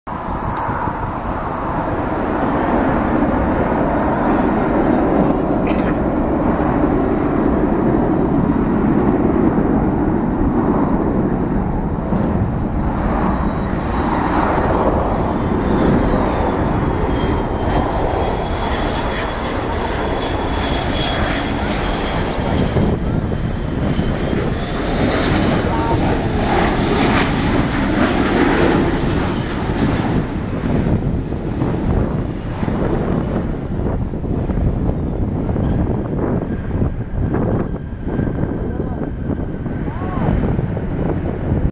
American Airlines McDonnell Douglas MD-83 [N965TW] landing in DFW
Flight info
Airline: American Airlines
Aircraft: McDonnell Douglas MD-83
Reg: N965TW
Livery: Normal
Airport: Dallas Fort Worth International Airport
Runway: 18R
Flight #: AA178
Route: ICT-DFW
Date: 07.26.19
Time: 5:19PM
Comments: Flew 965 XNA-DFW back in 12/2018.